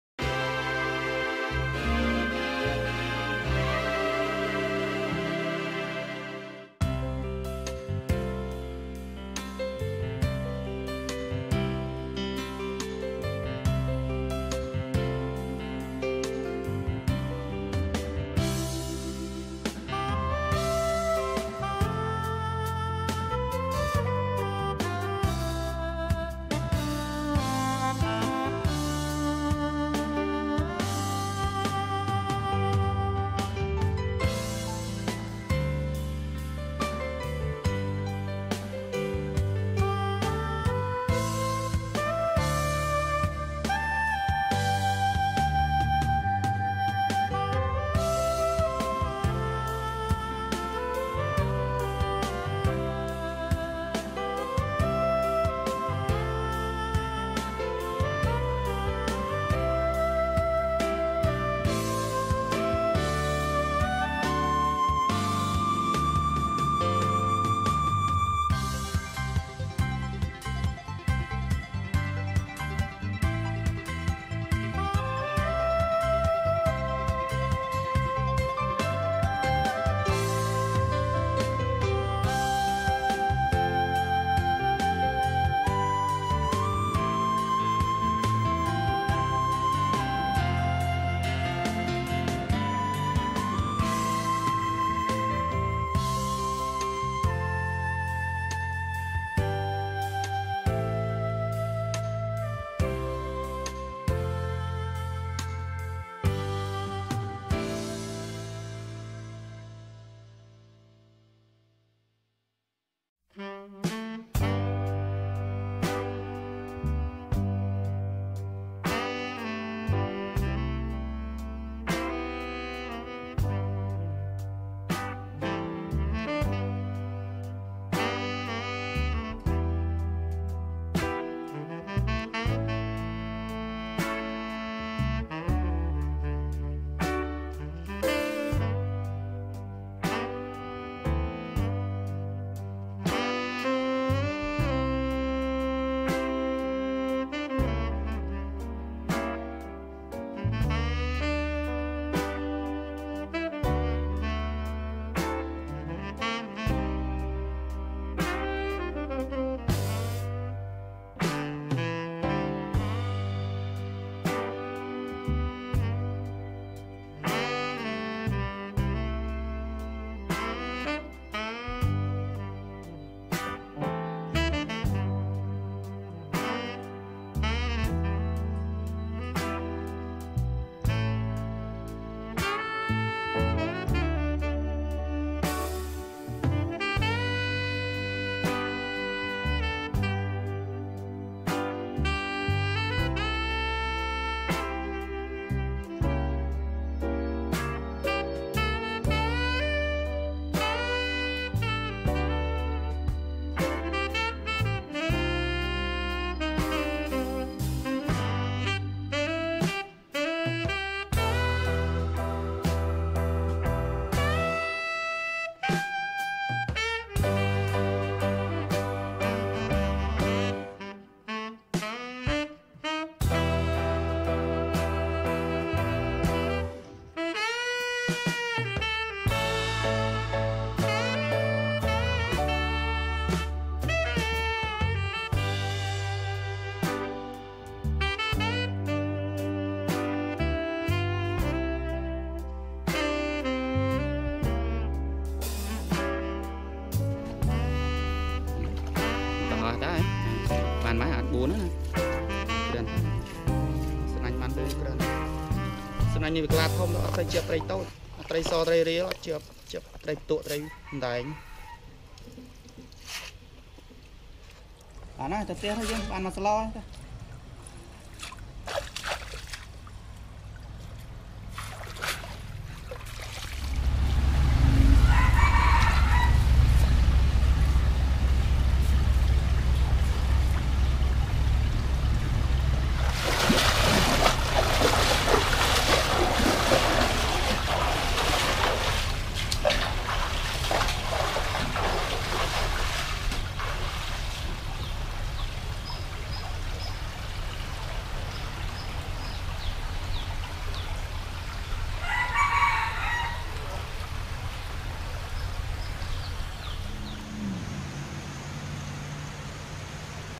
How To Catch Fish Using Stones And Cast Net -Cambodia Traditional Fishing
Hi! This Is Video I Want To Show You About How To Catch Fish Using Stones And Cast Net Date: (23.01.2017) -Cambodia Traditional Fishing.